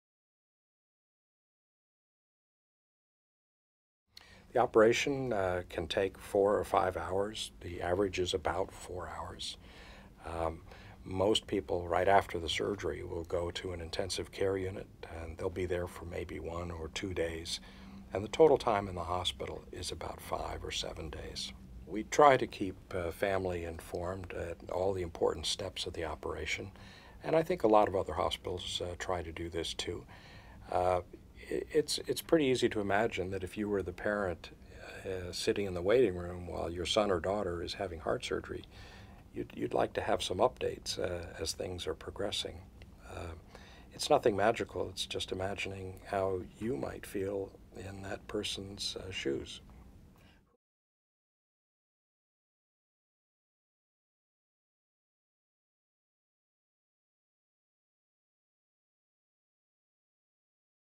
How long does the aortic surgery take?
Duke E. Cameron, MD, is the Cardiac Surgeon-in-Charge and the James T. Dresher Sr. Professor of Surgery at the Johns Hopkins Hospital, and the Director of the Dana and Albert "Cubby" Broccoli Center for Aortic Diseases at Hopkins. A member of our Professional Advisory Board, Dr. Cameron specializes in valve-sparing aortic root surgery.